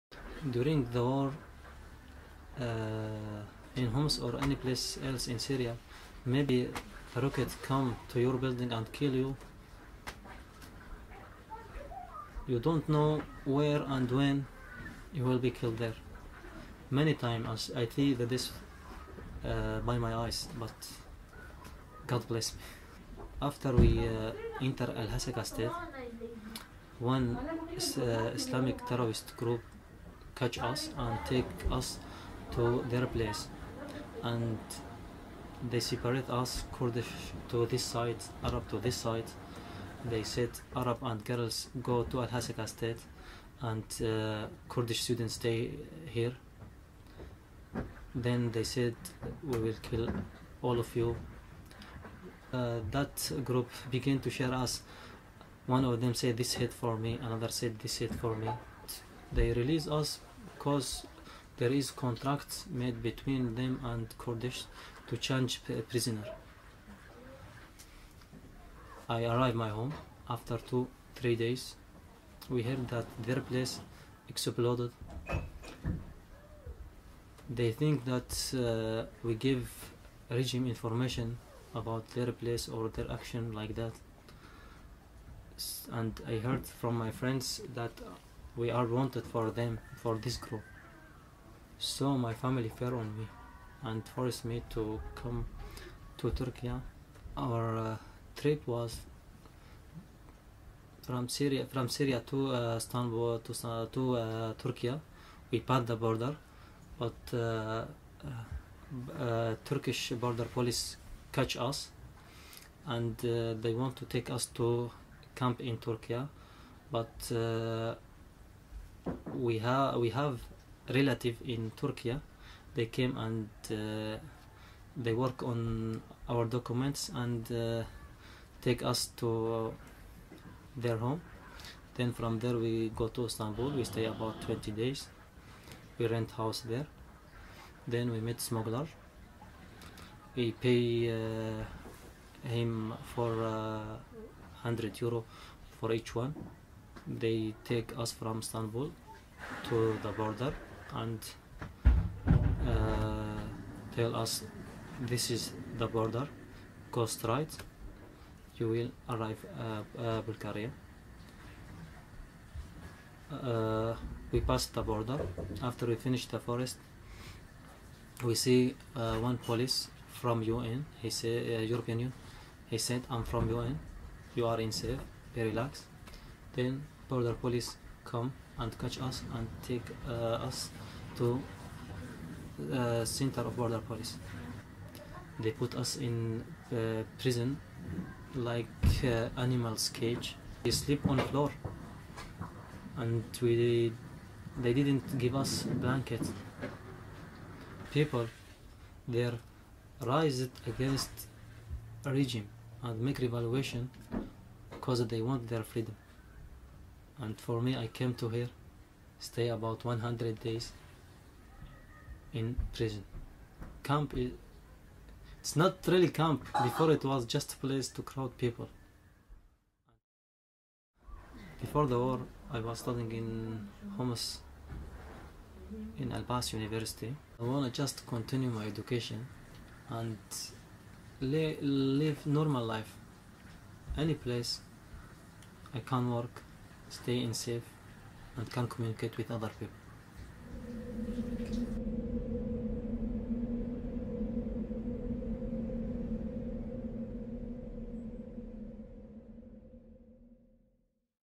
Mohamad in Bulgaria
Mohamad is a 24 year old student from Syria. Mohamad had to flee after he and his friends were captured while travelling back from university and threatened to be killed while in captivity only because they were Kurdish. He was released but was still in danger so he fled with his mother and brother and sisters to Bulgaria where he was detained upon arrival and had to live in an unheated tent. Mohamad dreams of continuing his education.
Take action to HelpSyriasRefugees - invite your family, friends and networks to sign the Europe Act Now petition